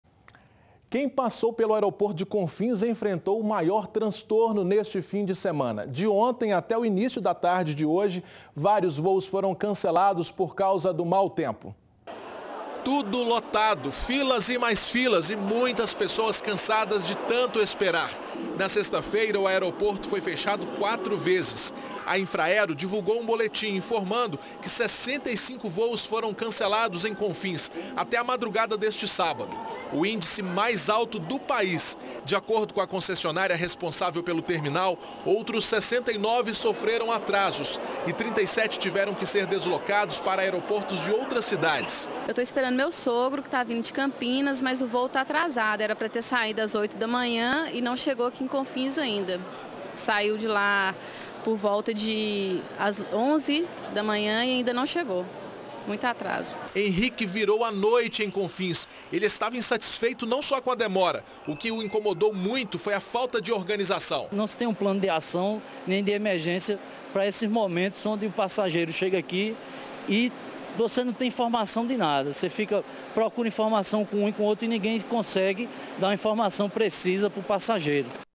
Diversos voos são cancelados devido mau tempo no Aeroporto de Confins
Quem passou pelo Aeroporto de Confins enfrentou o maior transtorno neste fim de semana. De ontem até o início da tarde de hoje, vários voos foram cancelados por causa do mau tempo.